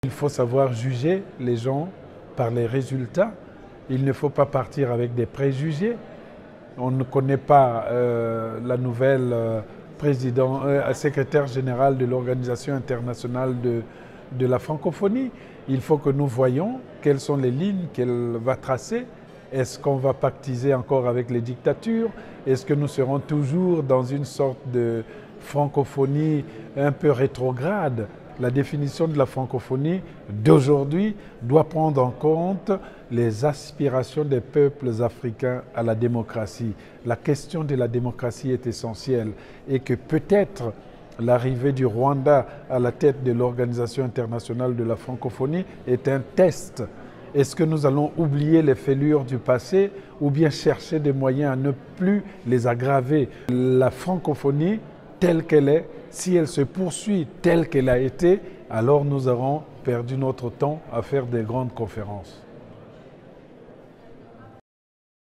La Francophonie doit mettre la démocratie au cœur de son action
Alain Mabanckou réagit à la désignation de la nouvelle secrétaire générale de la Francophonie, la Rwandaise Louise Mushikiwabo. L'écrivain franco-congolais est toujours aussi critique vis-à-vis de l'OIF mais espère que cette nomination placera la démocratie au cœur des actions de la Francophonie.